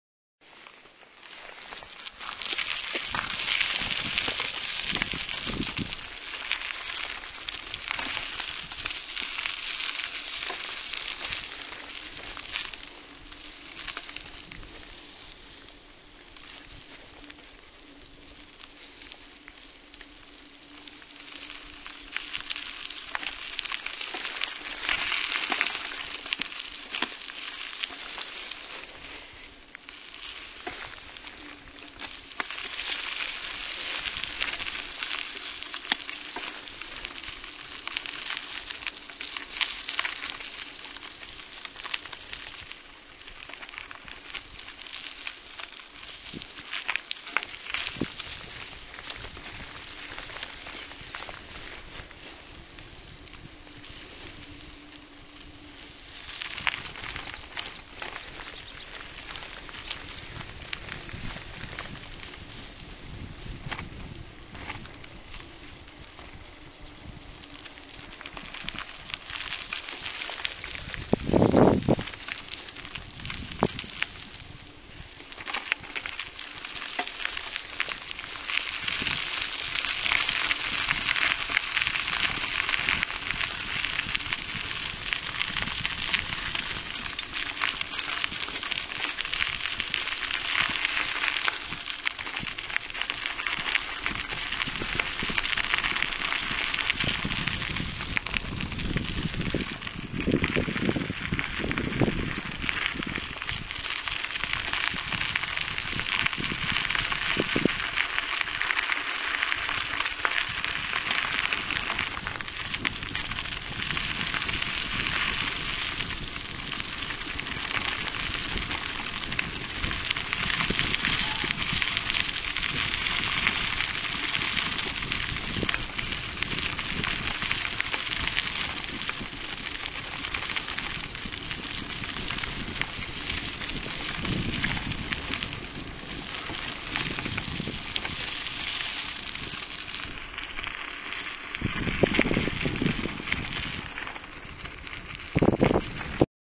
Промысла.
Еду на велосипеде выезжаю к посёлку промысла.